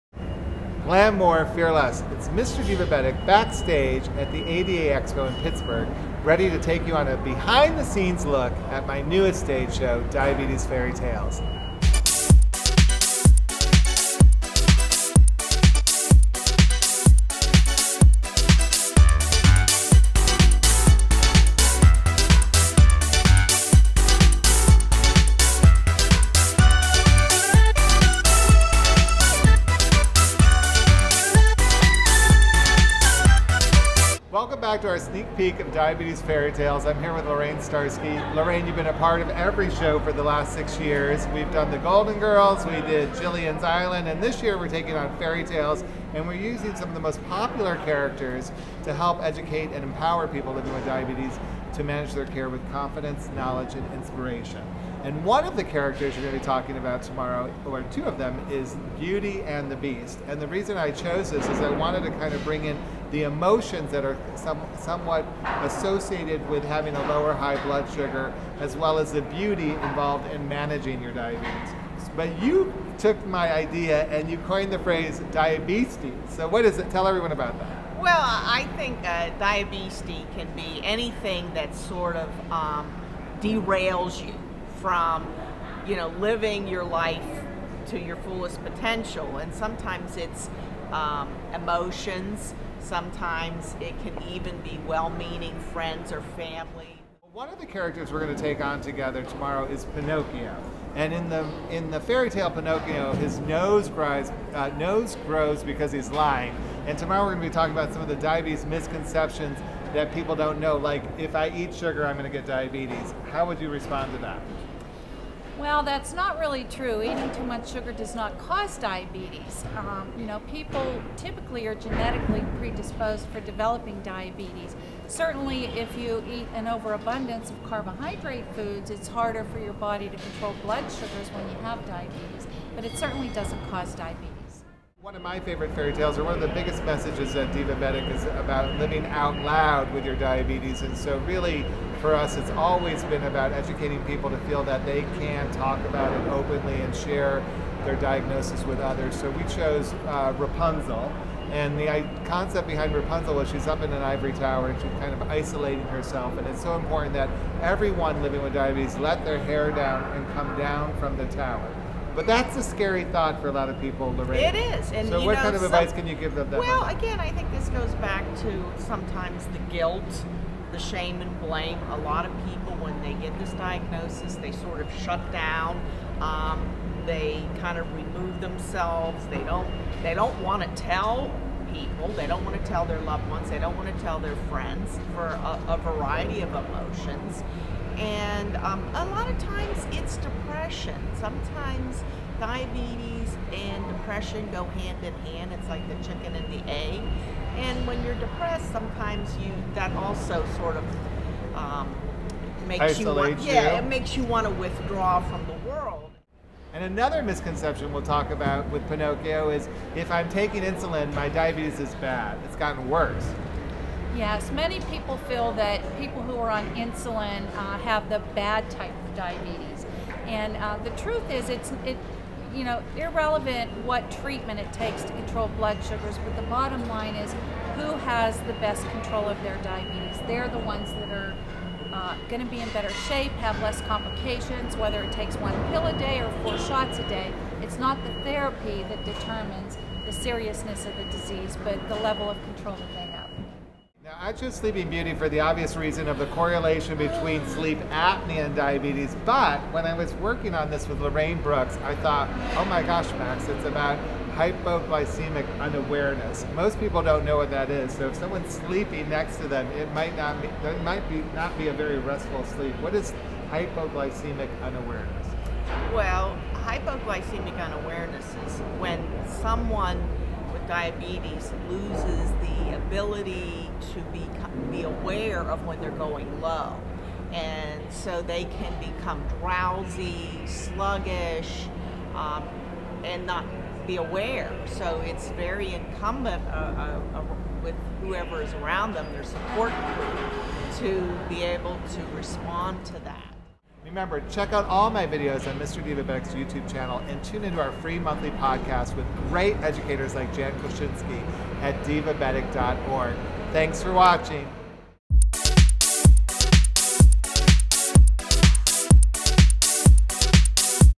Diabetes Fairy Tales
The educators with a little help from several popular characters such as Snow White, Pinocchio, Beauty & The Beast, Hansel & Gretel and Little Red Riding Hood brought  to life the American Association of Diabetes Educators (AADE) seven self-care behaviors: healthy eating, being active, monitoring, taking medication, problem solving, and reducing risks and made these topics more appealing to kids of all ages.

For example, the character of Rapunzel was used to personify the common feelings of isolation when first diagnosed with diabetes. Support meetings and educational classes are described as tools to escape a life living in a lonely tower by 'letting your hair down' and talking about your diabetes.

Results showed that diabetes education presented in this colorful and stylish manner have a positive impact on problem solving, reducing risky behaviors in type 2 populations.